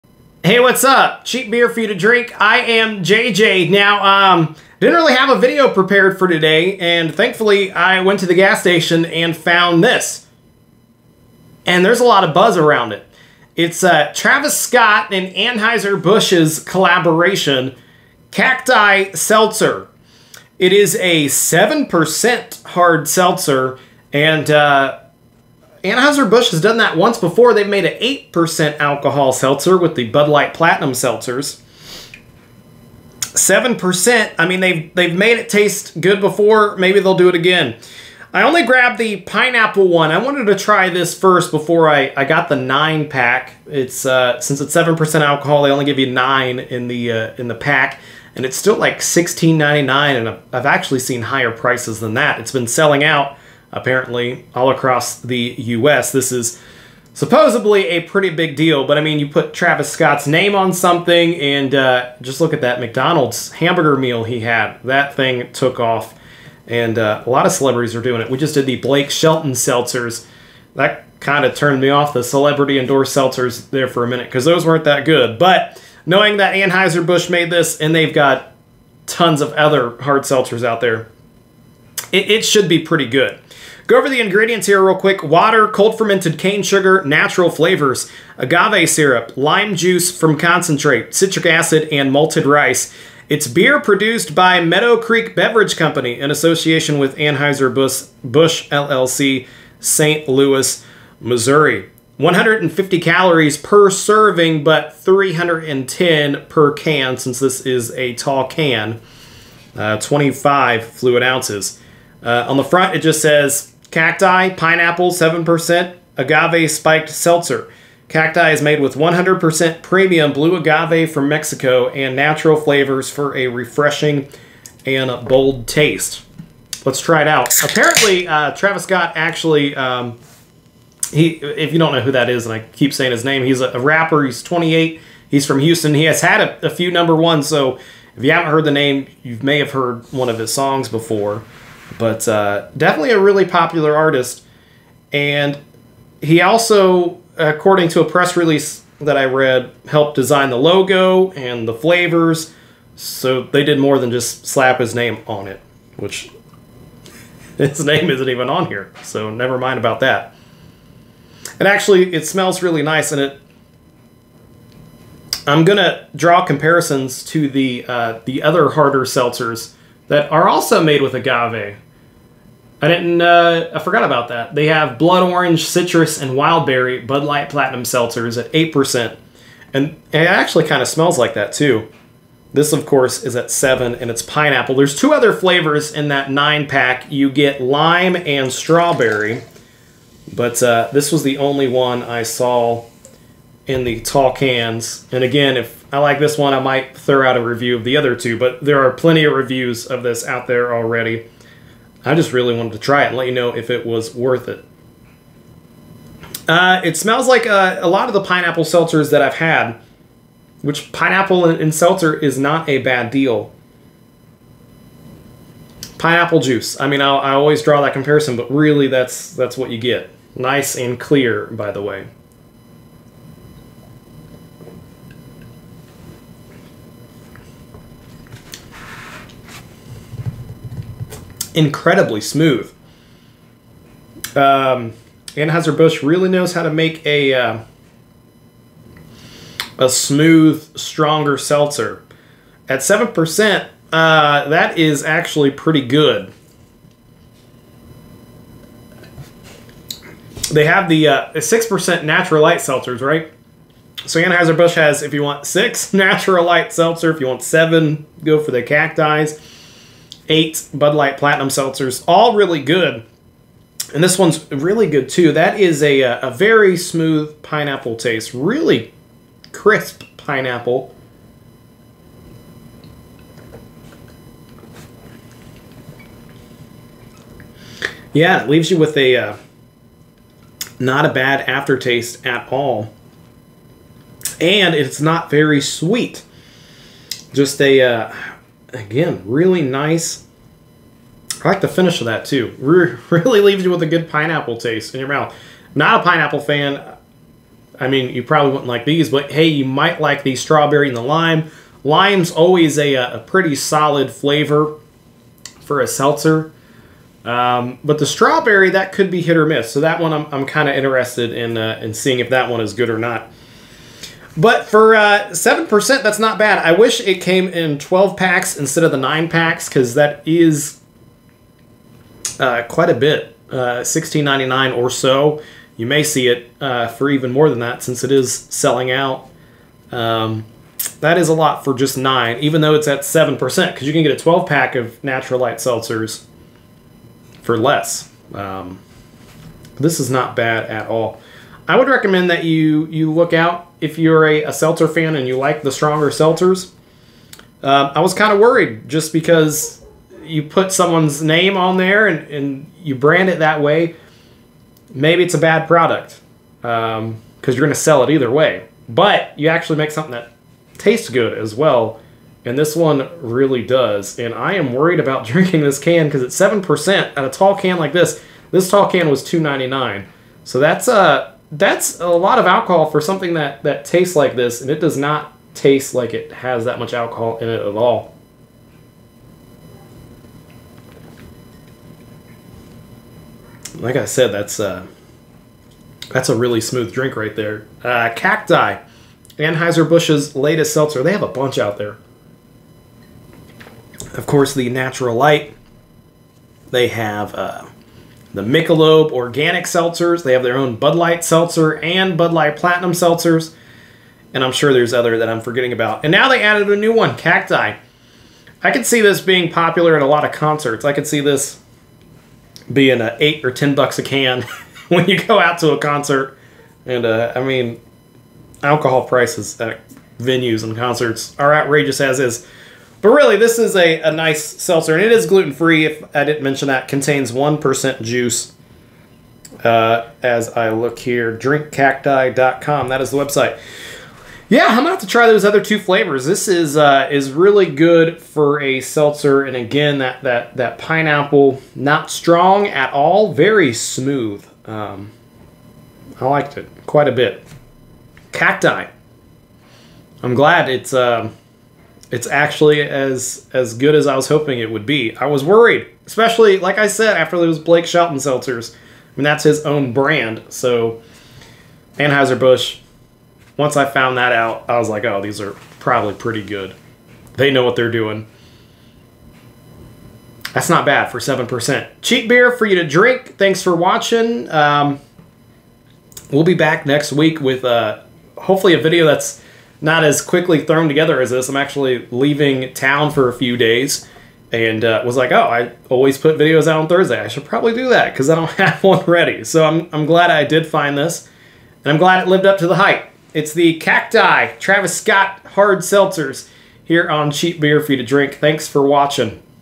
Cacti Agave Spiked Seltzer - Does Travis Scott's Seltzer Live Up to The Hype?
If you've had Bud Light Platinum Seltzer, you've had something very similar to this.